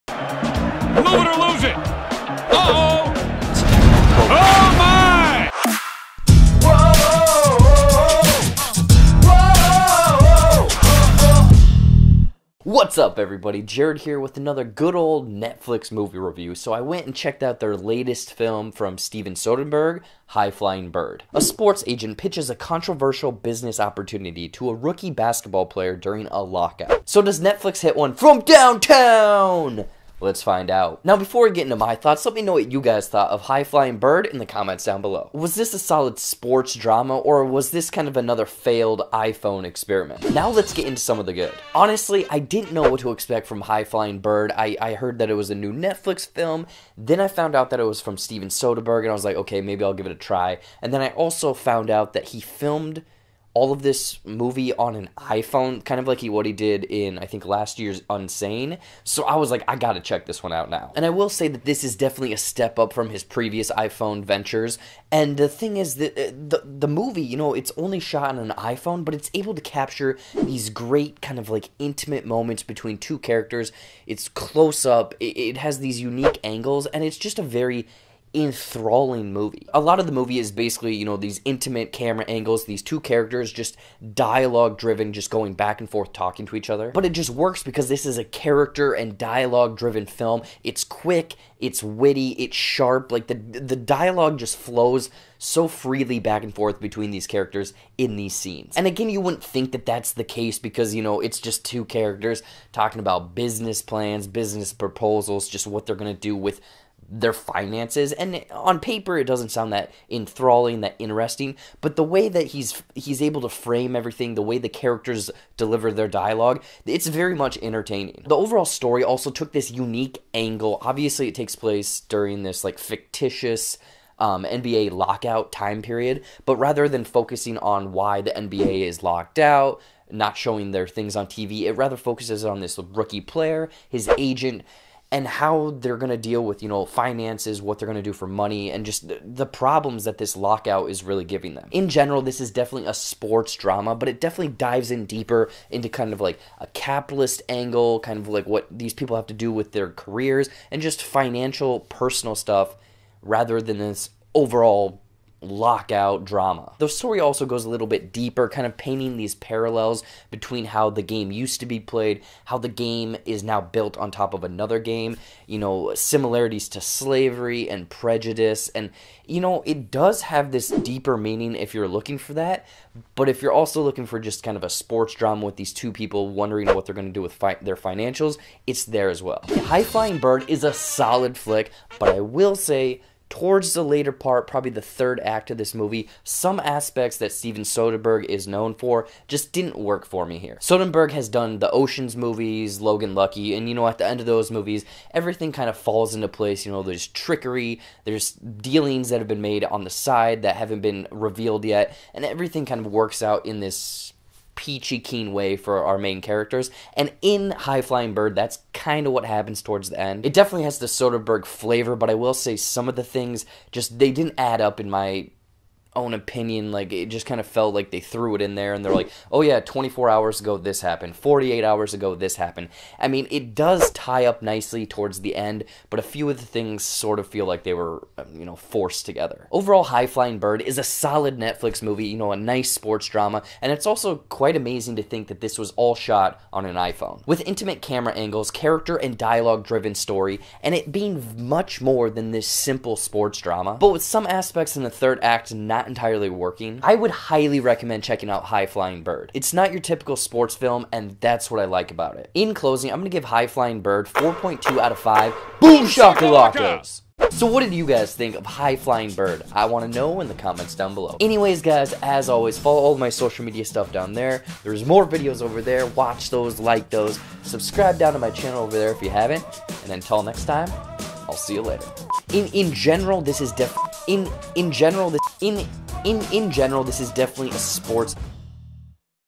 HIGH FLYING BIRD (2019) - Movie Review |Netflix Sports Drama
Netflix teams up with Steven Soderbergh for this NBA lockout sports drama, High Flying Bird. So does this new netflix flick sink nothing but net? Watch Jared's movie review to find out!

Plot Synopsis - High Flying Bird: A sports agent pitches a controversial business opportunity to a rookie basketball player during a lockout.

High Flying Bird is a 2019 American sports drama film directed by Steven Soderbergh, from a story suggested by André Holland, with the screenplay by Tarell Alvin McCraney.[3] The film stars Holland, Zazie Beetz, Melvin Gregg, Sonja Sohn, Zachary Quinto, Glenn Fleshler, Jeryl Prescott, Justin Hurtt-Dunkley, Caleb McLaughlin, Bobbi Bordley, Kyle MacLachlan and Bill Duke.

Director:
Steven Soderbergh

Produced by:
Joseph Malloch

Written by:
Tarell Alvin McCraney

Stars:
André Holland
Zazie Beetz
Melvin Gregg
Sonja Sohn
Zachary Quinto
Kyle MacLachlan
Bill Duke

Cinematography:
Steven Soderbergh
(as Peter Andrews)

Edited by:
Steven Soderbergh
(as Mary Ann Bernard)

Production Company:
Extension 765
Harper Road Films

Distributed By:
Netflix